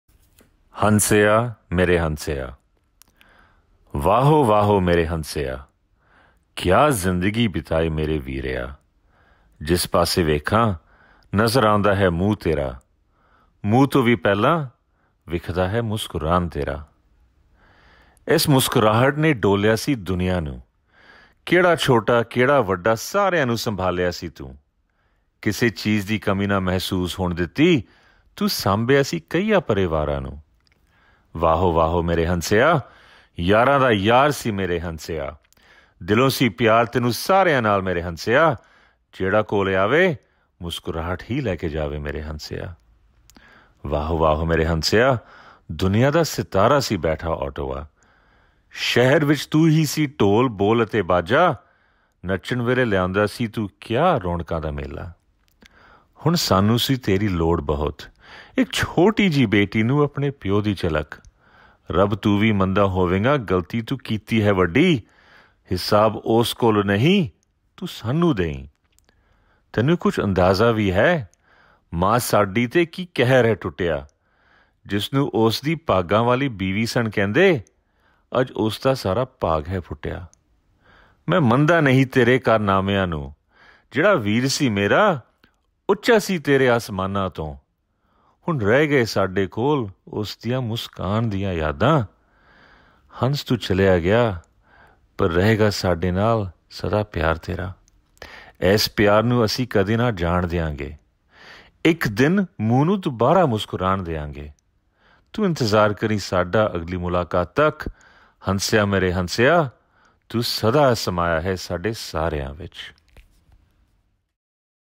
Hunseya Mere Hunseya 🧡💙❤ (Please click "play" for audio)
Love You Forever Huns 🧡💙❤ (Please click "play" for audio)
Poem recital by Bundeep Singh Rangar in loving memory of his dear brother Hansdeep Singh Rangar